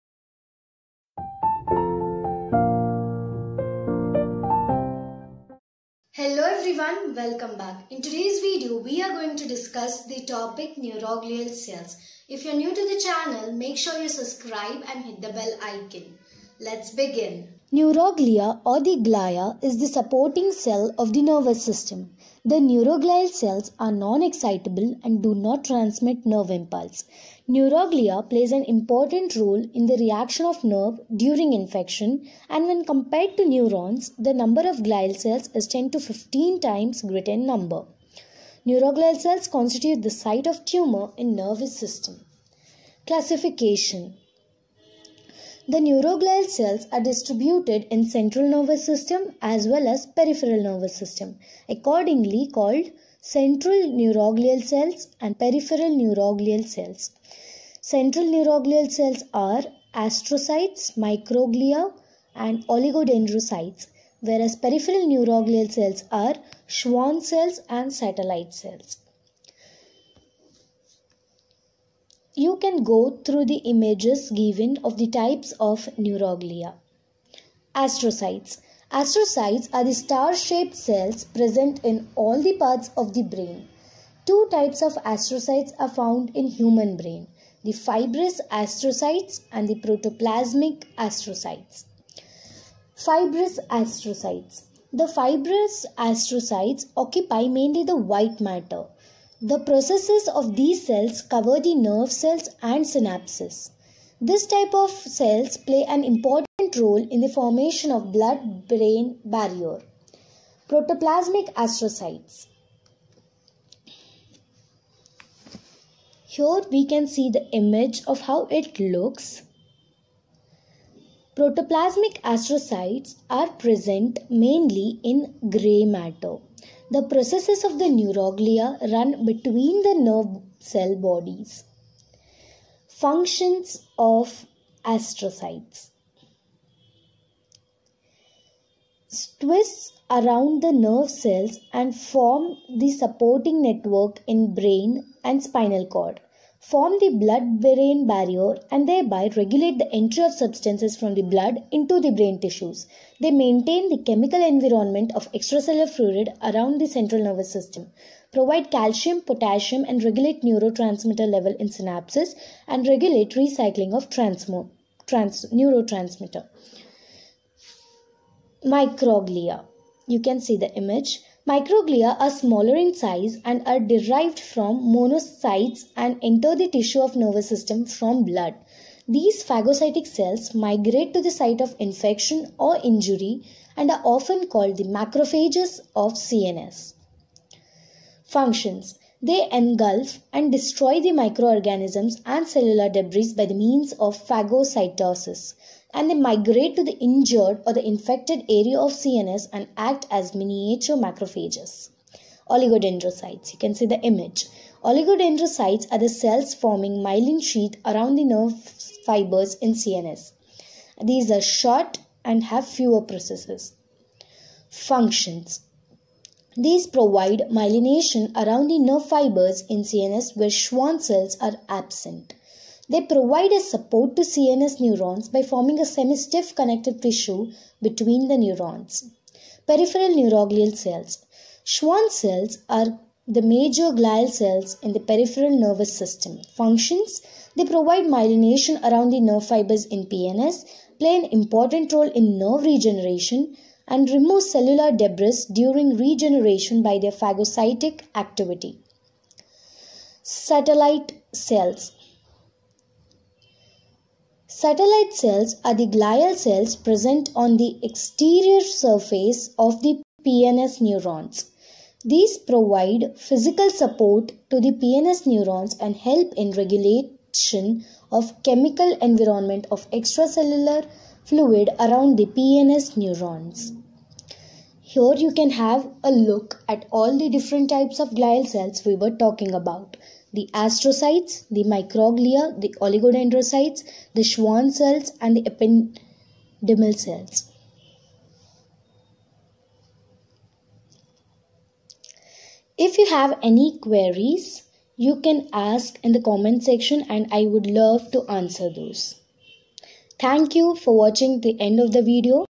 (Chapter 4) Neuroglial cells | Glial cells types | Neuroanatomy basics.
Neuroglial cells , Types of neuroglial cells or glial cells, thier functions, Neuroanatomy basics is explained in this video

Types of glial cells are divided into
Central neuroglial cells
Peripheral neuroglial cells